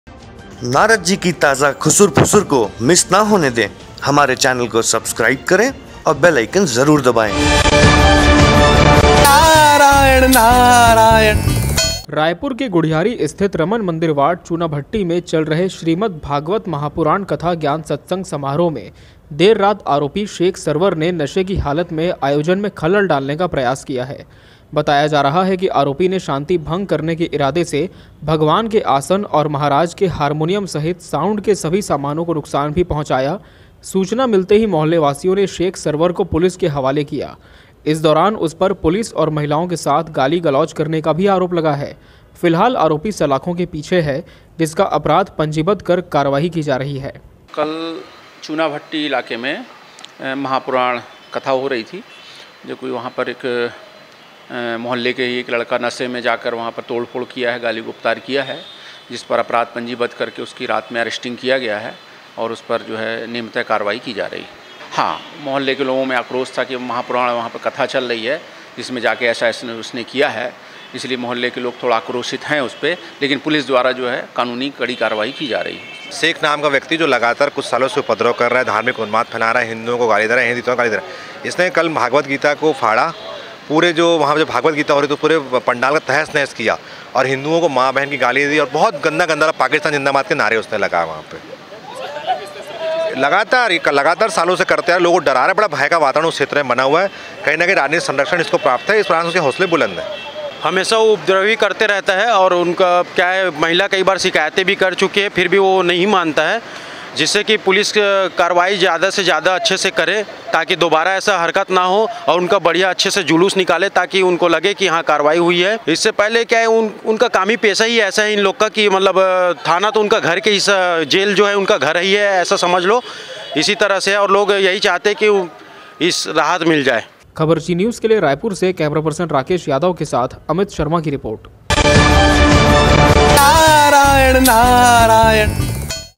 गुढ़ियारी इलाके में भगवत पाठ में खलल डालने वाला आरोपी गिरफ्त में
रायपुर के गुढ़ियारी स्थित रमन मंदिर वार्ड चूनाभट्टी में चल रहे श्रीमद भागवत महापुराण कथा ज्ञान सत्संग समारोह में देर रात  आरोपी  (शेख सरवर) ने नशे की हालत में आयोजन में खलल डालने का प्रयास किया है,बताया जा रहा है की आरोपी ने शांति भंग के इरादे से भगवान के आसन और महाराज के हारमोनियम सहित साउंड के सभी सामानों को नुकसान भी पहुँचाया है । सूचना मिलते ही मोहल्लेवासियों ने शेख सरवरको पुलिस के हवाले किया । इस दौरान उसपर पुलिस और महिलाओं के साथ गाली गलौज करने का भी आरोप लगा है,फिलहाल आरोपी सलाखों के पीछे है जिसका अपराध पंचिबद्ध कर  कार्यवाही की जा रही है.